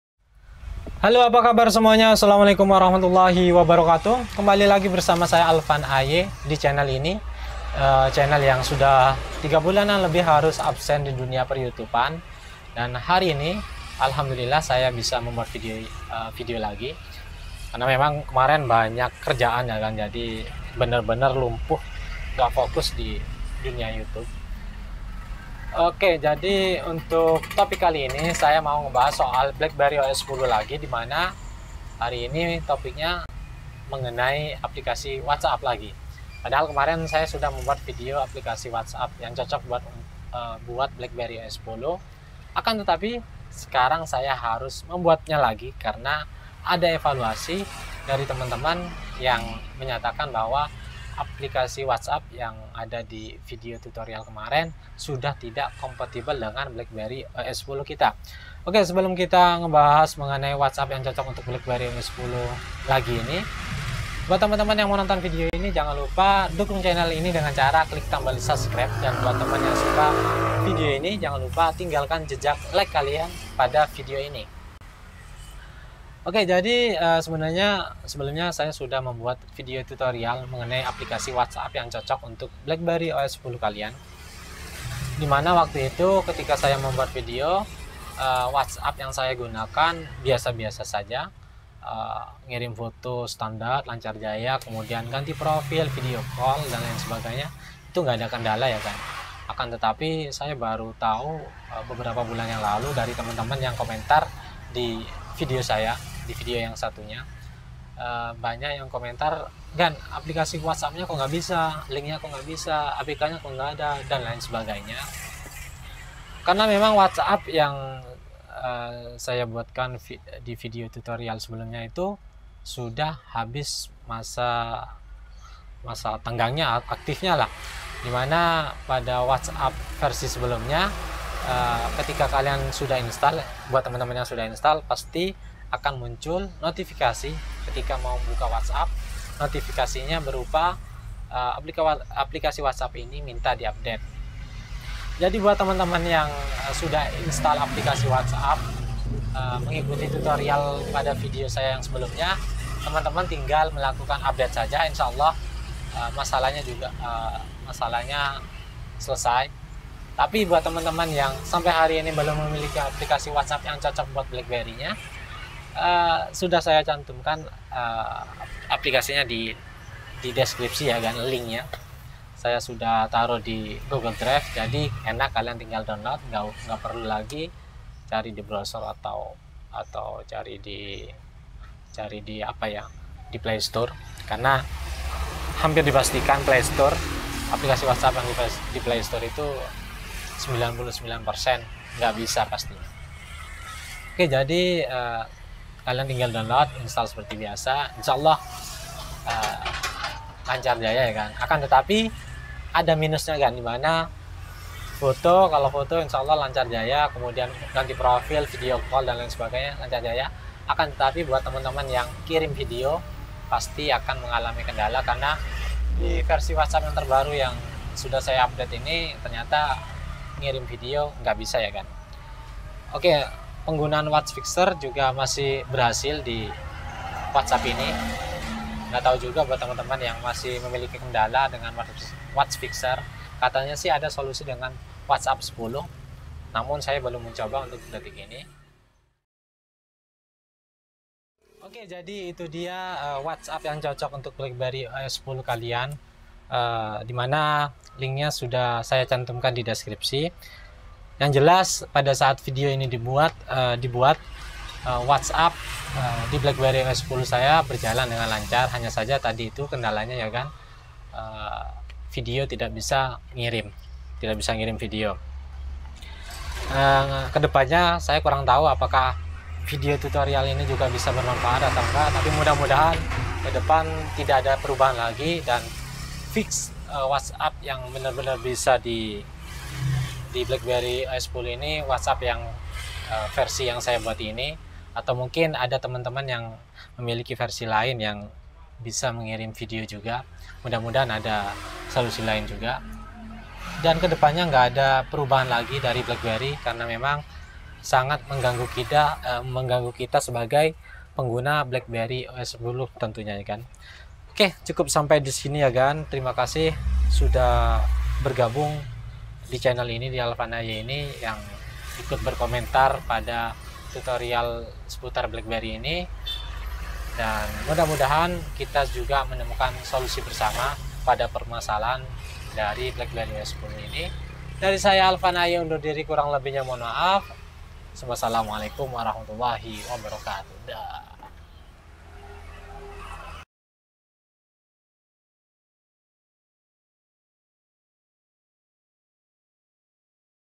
Terbaru !!! Akhirnya Whatsapp ini yang cocok di Blackberry OS 10
Halo semuanya buat agan agan
Sebelumnya Alfan ay mau mengucapkan mohon maaf lahir batin yah 😂

Lama sekali rasanya g upload video
Sudah 3 bulanan lebih saya g upload video. Dan baru ini Alhamdulillah akhirnya sempat buat video .

Adapun topik yang akan kita bahas kali ini yaitu mengenai WhatsApp lagi y gan 😁

Gak bosan bosannya bahas whatsapp di BlackBerry ini.
Ada aja masalah yang timbul
Salah satunya juga g ada Aplikasi whatsapp yg compatible dengan blackberry kita semenjak tanggal 31 Desember 2017 whatsapp meninggalkan OS ini

Oke , jadi sebelumnya saya membuat tutorial mengenai Aplikasi whatsapp yang cocok buat BlackBerry Os10
Waktu itu juga banyak yang berhasil mengikuti tutorial tersebut, Namun belakang an ini banyak temen temen yang mengeluh ternyata tutorial yang saya buat sebelumnya sudah gak bisa diterapkan di BlackBerry OS 10 ini gan 😂

Oke langsung saja ikuti tutorial di video ini y gan
Akhirnya Whatsapp versi ini yang bisa di BlackBerry OS 10